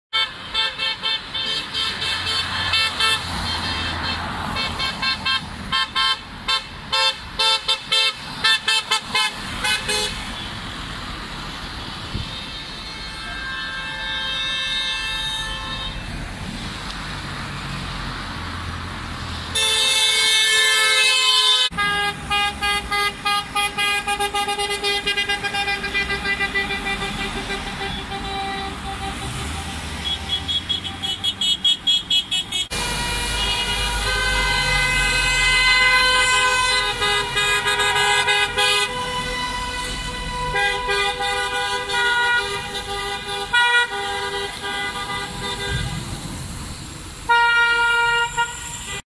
Manifestación en contra de la LOMLOE
Manifestación de vehículos en Pamplona, convocada por la plataforma 'Más Plurales', en contra de la LOMLOE, la reforma educativa conocida como 'Ley Celaá'